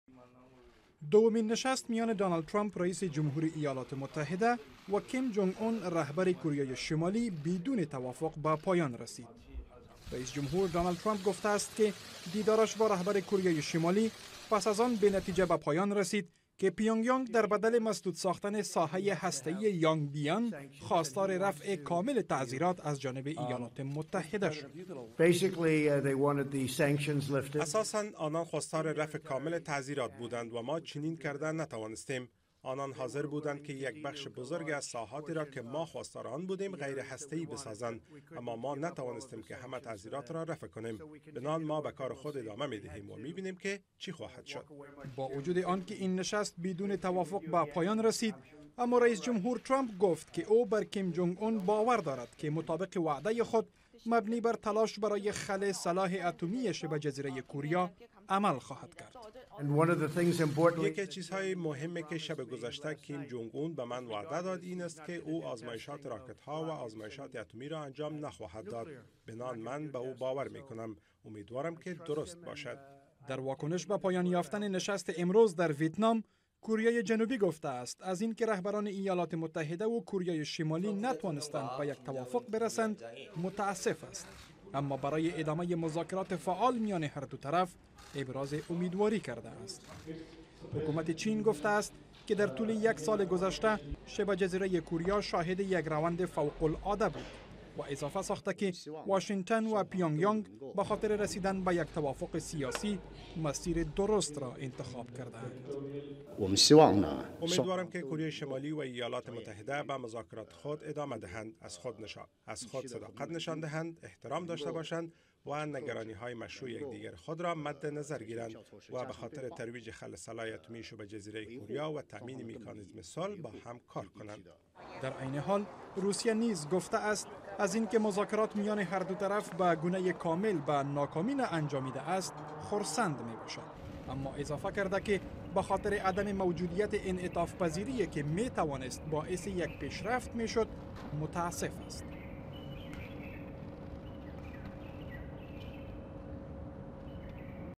Trump Kim meeting reactions - VOA Ashna
نشست میان دونالد ترمپ، رئیس جمهور ایالات متحده و کیم جونگ اُون، رهبر کوریای شمالی، بدون توافق به پایان رسید. رئیس جمهور ترمپ دلیل این امر را تقاضای بیش از حد کوریای شمالی خوانده است. دیدار دو روزه میان هر دو رهبر، دیروز در هانوی، پایتخت ویتنام آغاز شده بود.